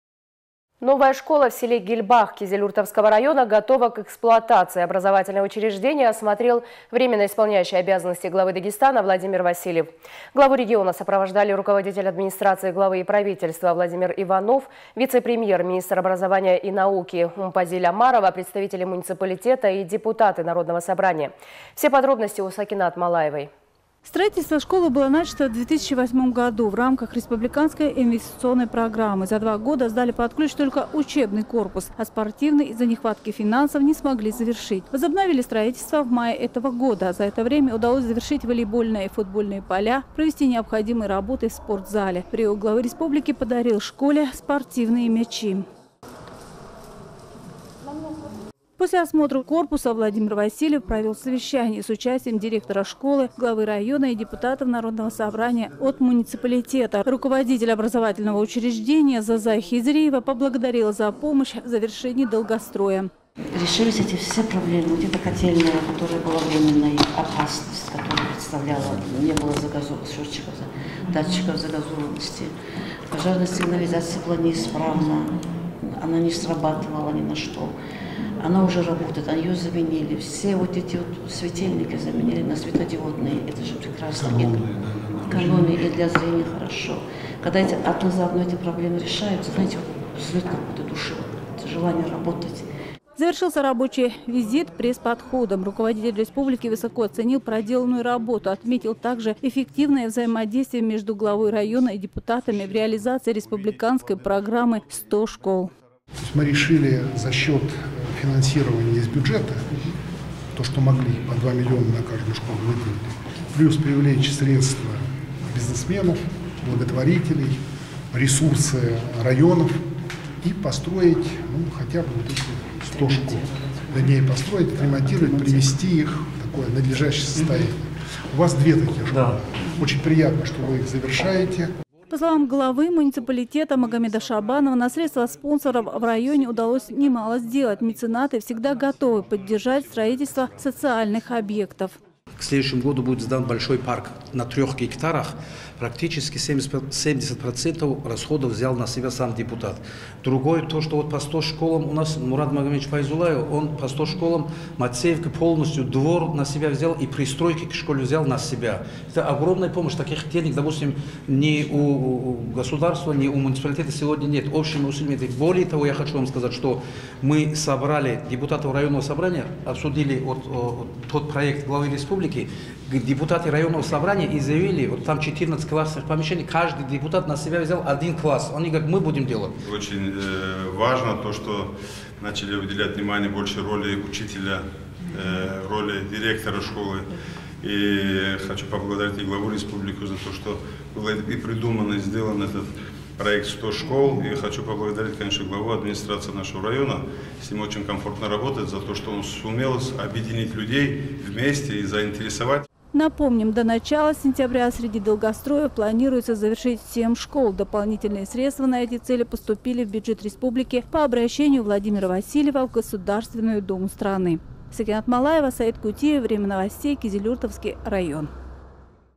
Школа в с. Гельбах готова к эксплуатации. Завершенный долгострой осмотрел врио главы Дагестана
Телеканал РГВК Дагестан, сюжет С.Маллаевой
Новая школа в селе Гельбах Кизилюртовского района готова к эксплуатации. Образовательное учреждение осмотрел врио главы Дагестана Владимир Васильев. Главу региона сопровождали руководитель Администрации главы и правительства Владимир Иванов, вице-премьер, министр образования и науки Уммупазиль Омарова, представители муниципалитета и депутаты Народного Собрания.